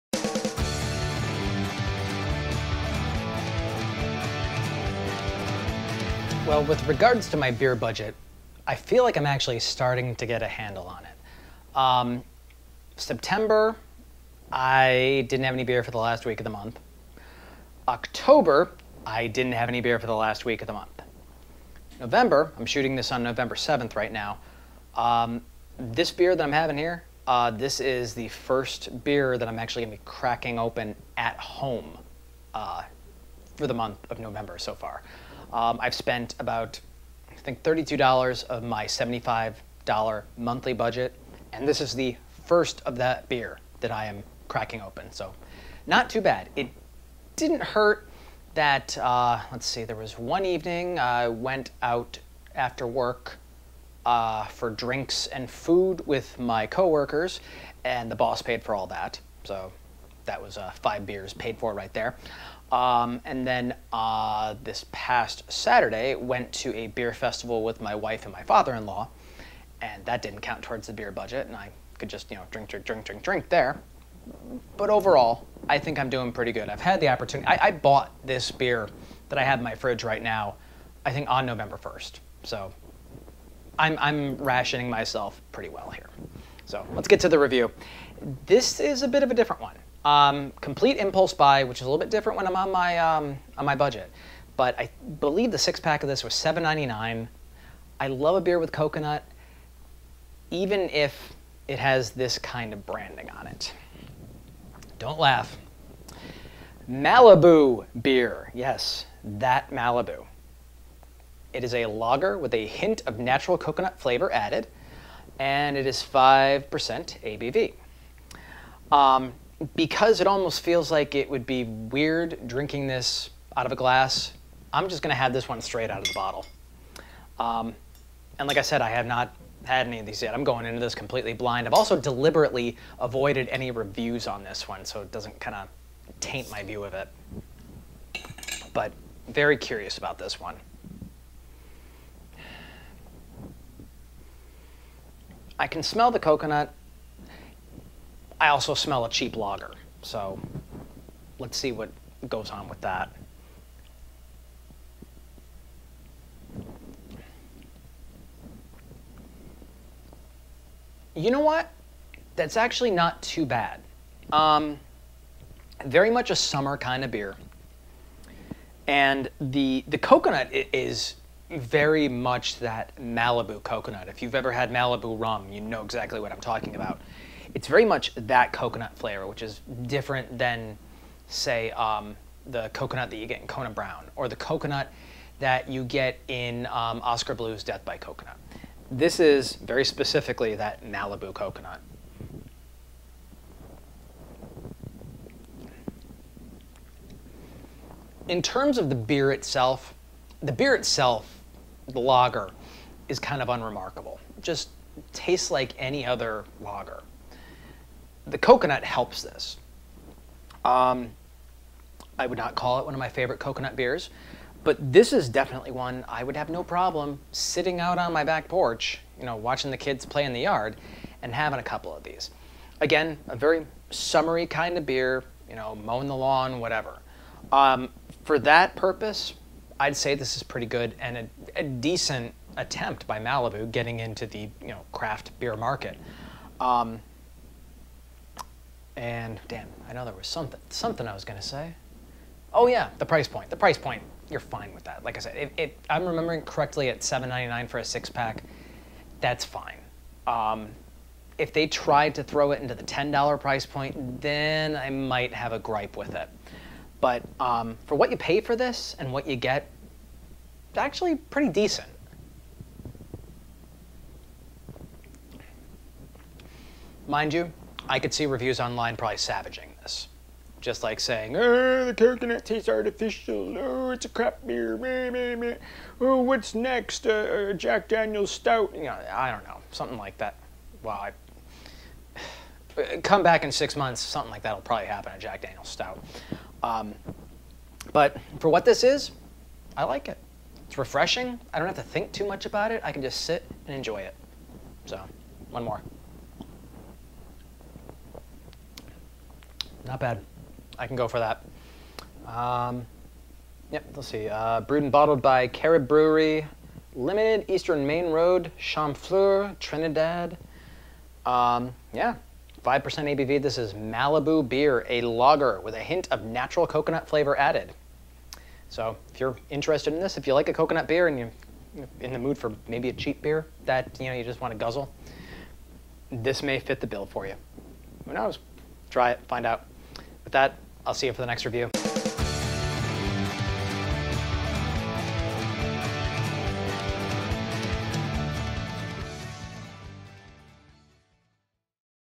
Alcohol By Volume, Review 170. Malibu Beer
Looks like the distilleries are getting into the beer-making game. Can Malibu bring their signature coconutty goodness to a beer? It's Malibu Beer!